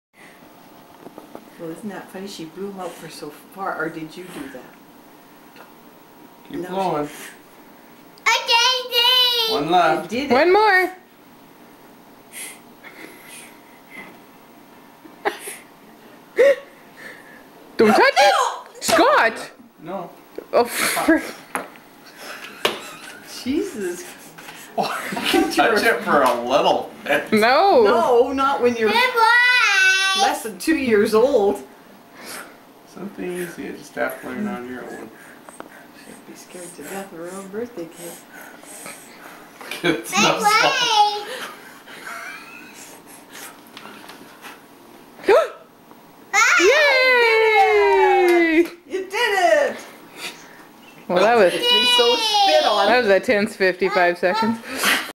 Happy Birthday Scott
Scott's 31st birthday.  Julia helping him blow out the candles.